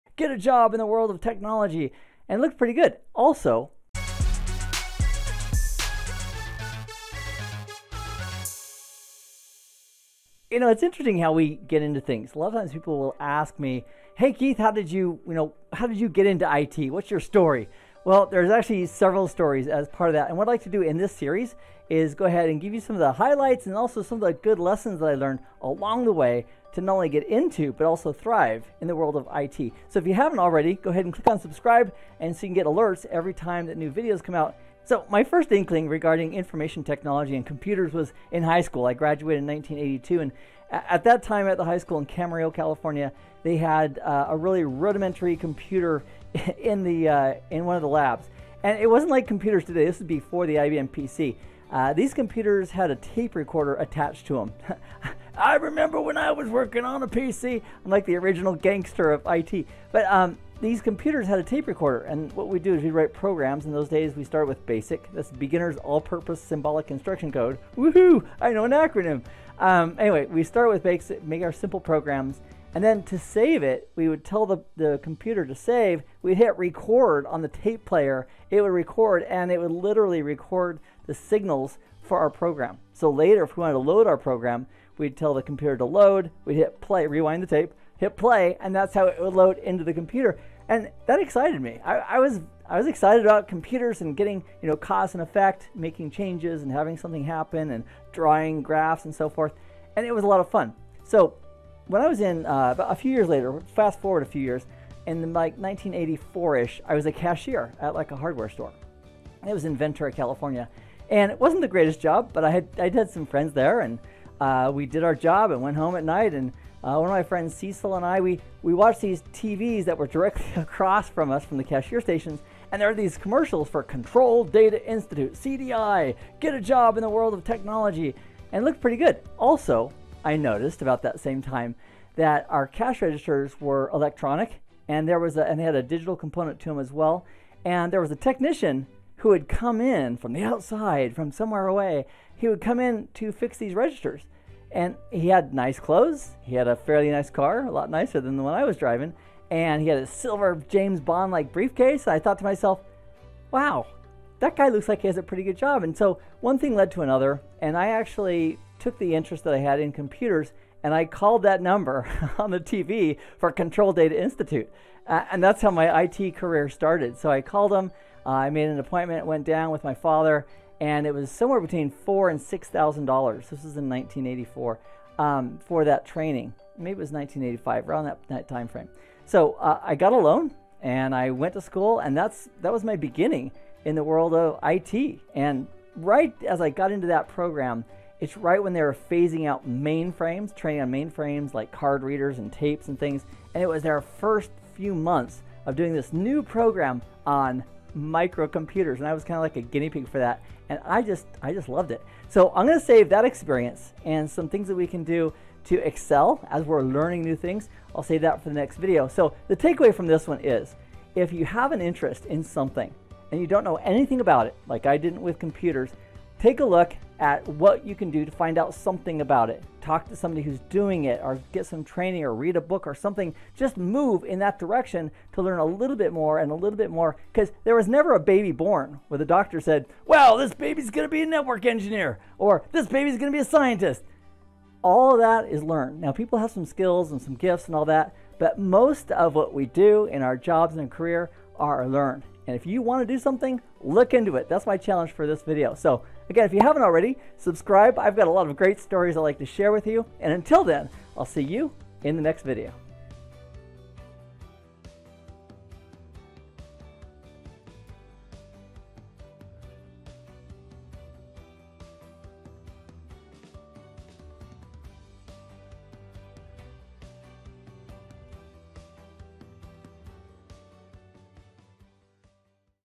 How I Got Started in the IT Industry | What was it like in the 80s?
Do you ever wonder what IT was like as an industry when it first started? I have been there since the very beginning, and I'd like to share with you my story, and my experience. Do you want a career in IT? Are you looking to find out more about the industry and stay up to date on everything new that comes out in the world of tech, networking, and security? I can help!

I've been in the world of IT learning, teaching, and gathering knowledge since the mid 80's. In this video I'd like to share with you my start, and how I got into IT just as the ball started rolling. You can go ahead and call me the OG of IT, because there really aren't that many people still doing this that have been in the industry for as long as I have.

I have always been so interested in the way computers worked, even back in the day when the technology was first becoming more advanced. It was completely different back then, and I've been on the edge of my seat, following the newest and latest developments in the industry ever since.

I believe that anyone can change their station in life, by including the habits and actions (some small, and some big), that lead them in the direction of their desires. I am living proof. Anyone can do it.